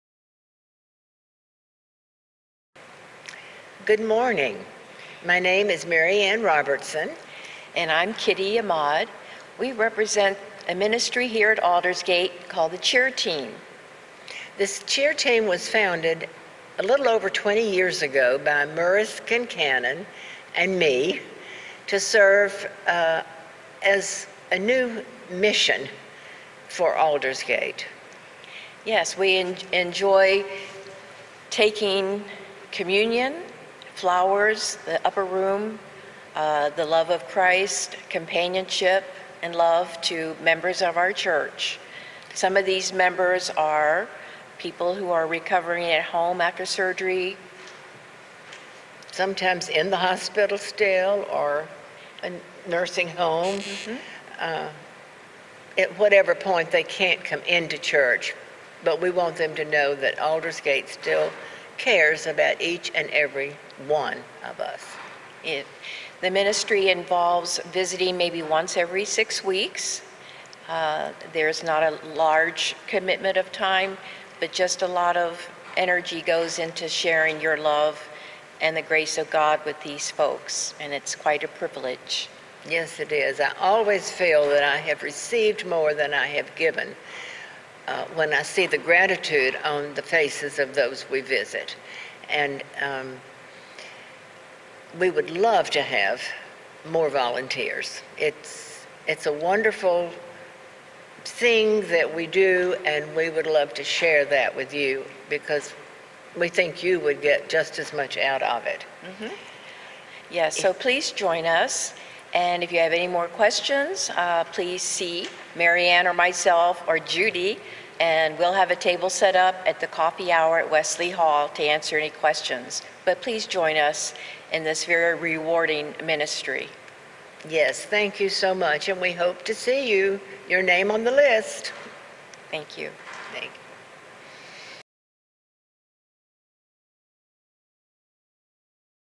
Aldersgate UMC Cheer Team, Ministry Moment (February 2024)
Join Kitty Ahmad and Mary Ann Robertson as they share experiences serving and working in our community as a part of the Aldersgate UMC CHEER Team.